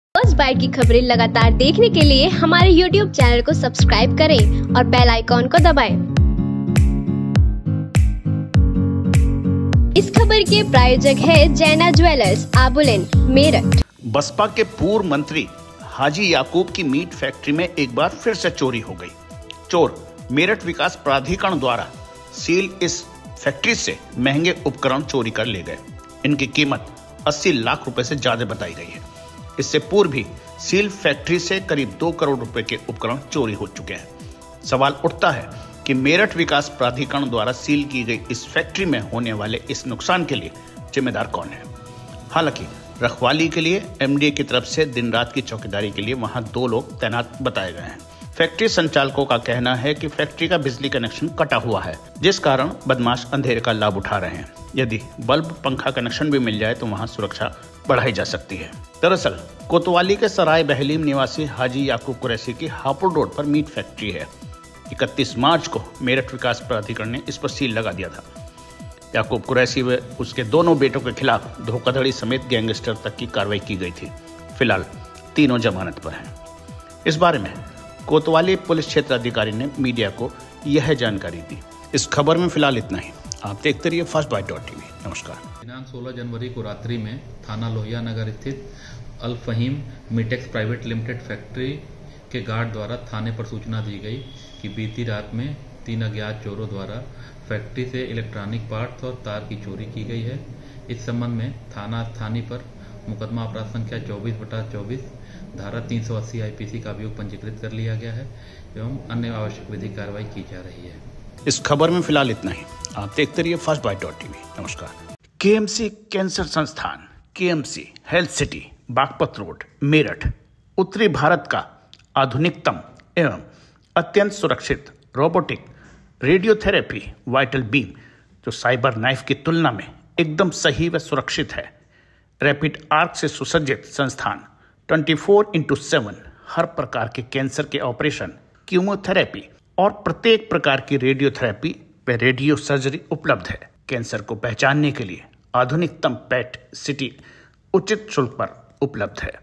सील फैक्ट्री में अस्सी लाख के माल की चोरी के लिये जवाबदेह कौन ? - First Byte Tv
बसपा के पूर्व मंत्री याकूब कुरैशी की फैक्ट्री में फिर चोरी
इस बार अस्सी लाख के उपकरण चोरी
अब तक दो करोड़ के उपकरण हो चुके हैं चोरी
एमडीए की सील व चौकीदारी के बाद भी चोरी
लगातार हो रही चोरी..बड़ी साजिश या फिर कुछ और ?
बसपा के पूर्व मंत्री हाजी याकूब की मीट फैक्ट्री में एक बार फिर से चोरी हो गई। चोर मेरठ विकास प्राधिकरण द्वारा सील इस फैक्ट्री से महंगे उपकरण चुरा कर ले गये। इनकी कीमत अस्सी लाख रुपये से ज्यादा बतायी गई है। इससे पूर्व भी सील फैक्ट्री से करीब दो करोड़ के उपकरण चोरी हो चुके हैं। सवाल उठता है कि मेरठ विकास प्राधिकरण द्वारा सील की गई इस फैक्ट्री में होने वाले इस नुकसान के लिये जिम्मेदार कौन है। हालांकि रखवाली के लिये एमडीए की तरफ से दिन रात की चौकीदारी के लिये वहां दो लोग तैनात हैं। फैक्ट्री संचालकों का कहना है कि फैक्ट्री का बिजली कनेक्शन कटा हुआ है, जिस कारण अंधेरे का लाभ बदमाश उठा रहे हैं। यदि बल्ब पंखा कनेक्शन भी मिल जाये तो वहां सुरक्षा बढाई जा सकती है।
There was theft once again in the meat factory of former BSP minister Haji Yakub. Thieves stole expensive equipment from this factory sealed by Meerut Development Authority. Their price is said to be more than eighty lakh rupees. Even before this, equipment worth about Rs 2 crores had been stolen from the Seal factory. The question arises that who is responsible for this loss occurring in this factory sealed by Meerut Development Authority. However, for guarding, two people are deployed there on behalf of MDA for day and night vigil. Factory operators say that the electricity connection of the factory has been cut, due to which miscreants are taking advantage of the darkness. If bulb fan connection is also available then security can be increased there.